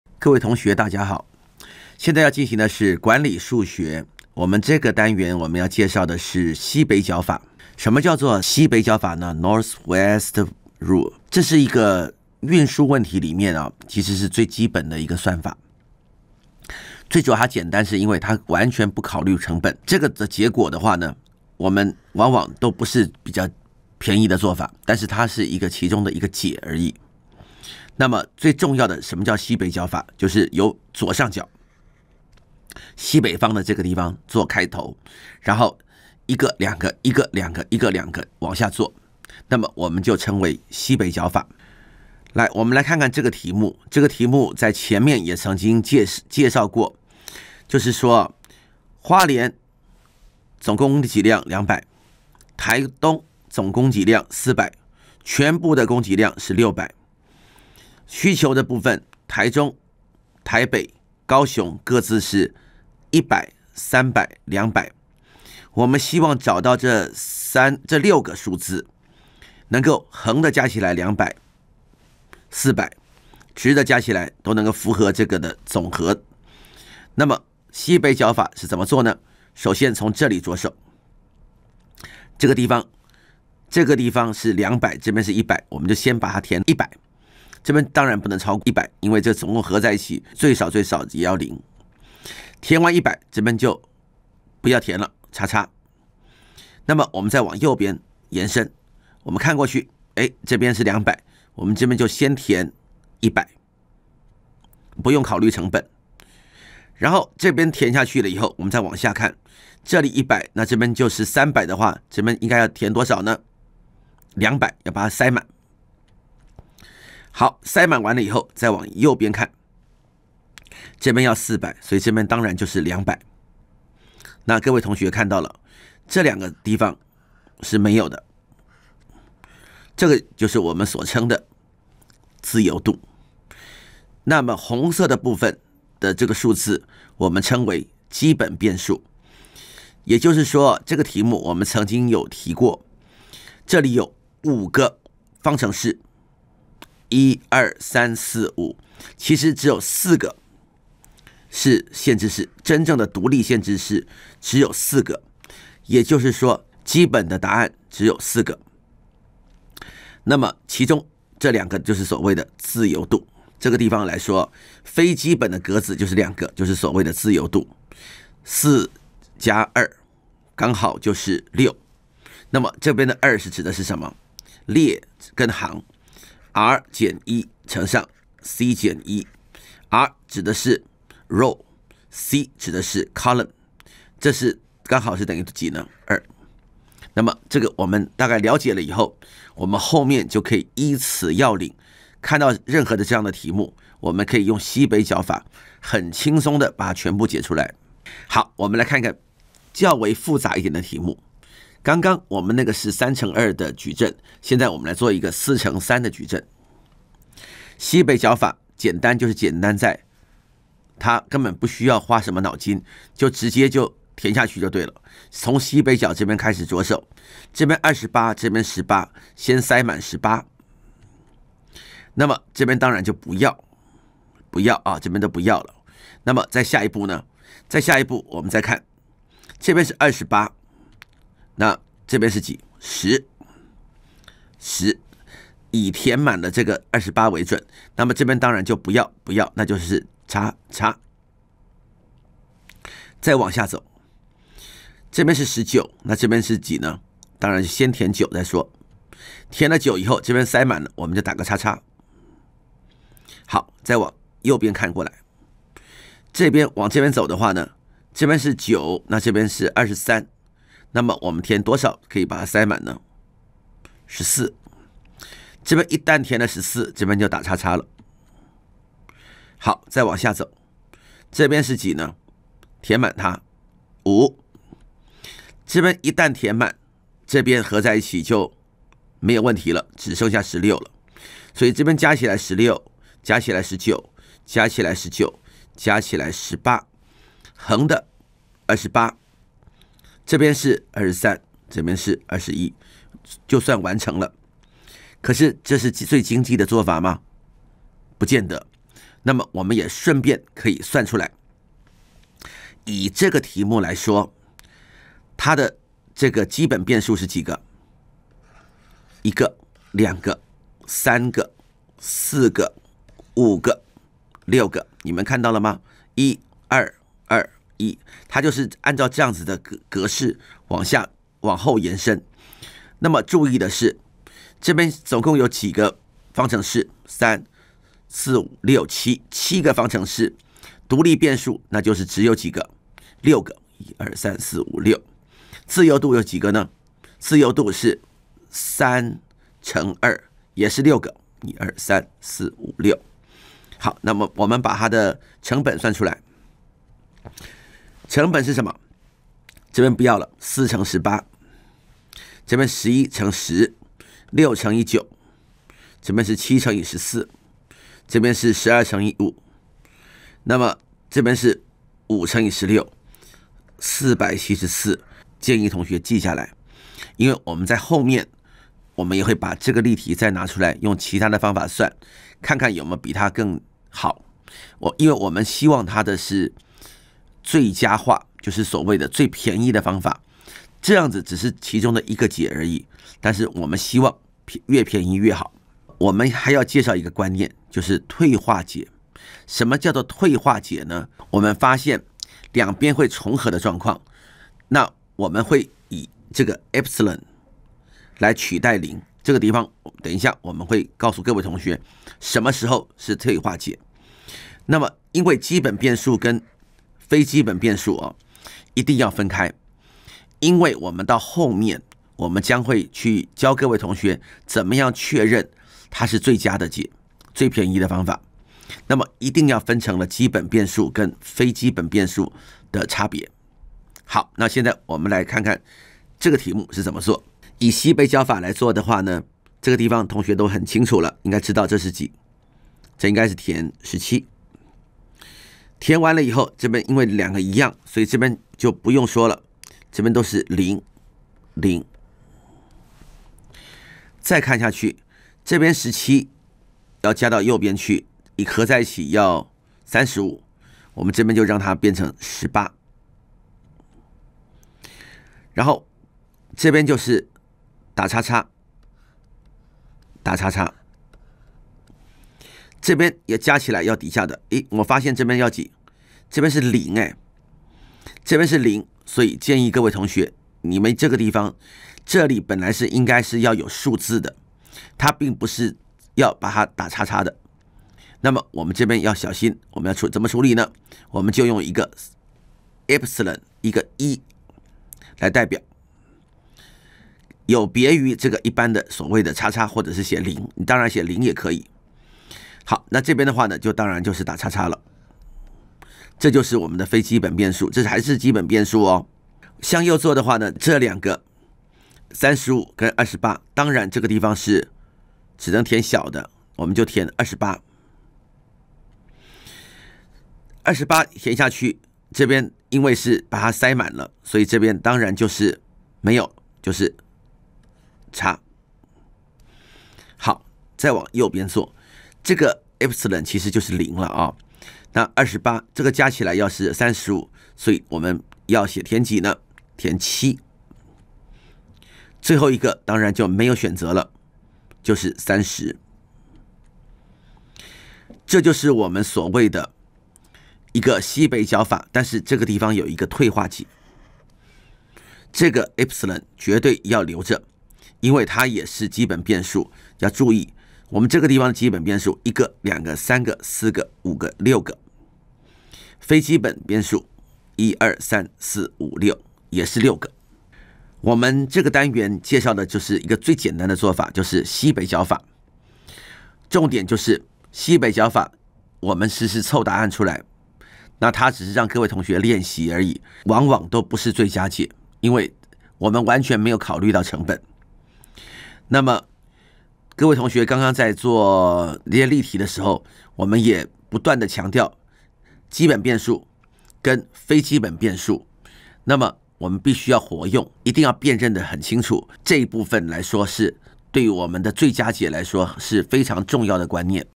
管理數學22-運輸問題-西北角法
課程簡介：以西北角法找出運輸問題的解
課程難度：■■□□□
適合對象：修過基礎數學的同學
授課教師：李柏堅
製作單位：中華科技大學 遠距教學組
製作人員：林文博